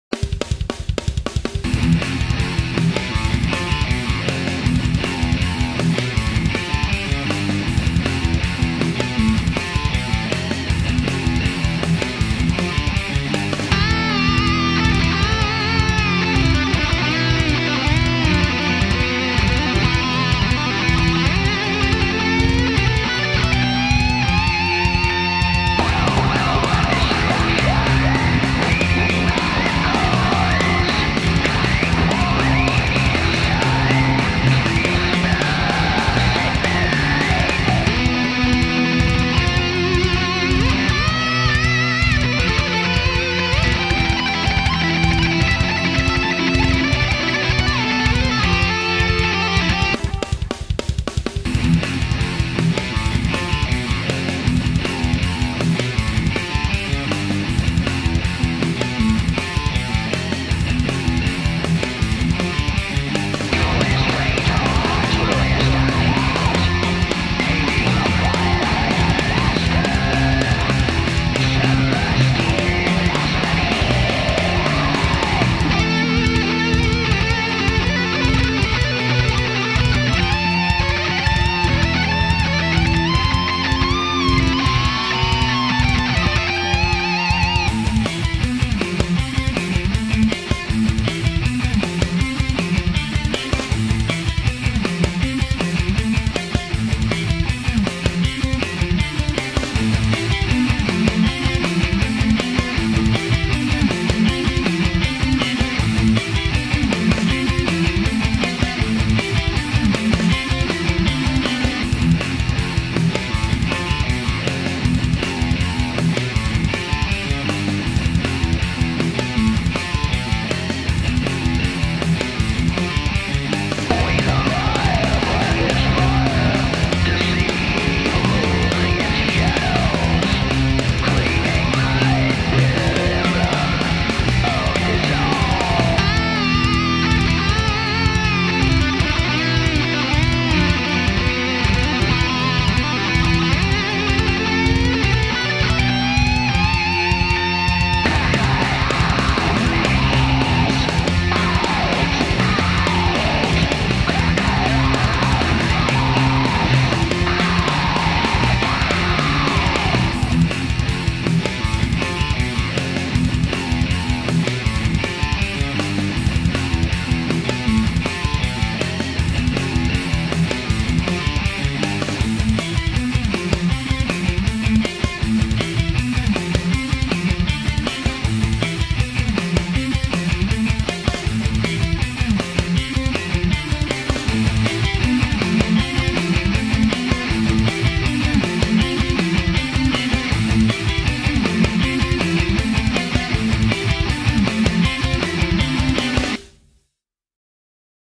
Moving Against Shadows (Leviathan Inspired)
After reading the great article on Jef Whitehead in the recent issue of Guitar World (Jun2015), I was inspired to record something similar to a Leviathan-like black metal tune.
New guitar book: Guitar Patterns, Licks, and More
Also check out "How to Become a Guitar Player from Hell" and the "Underground Guitar Handbook" at your favorite online book store, thanks.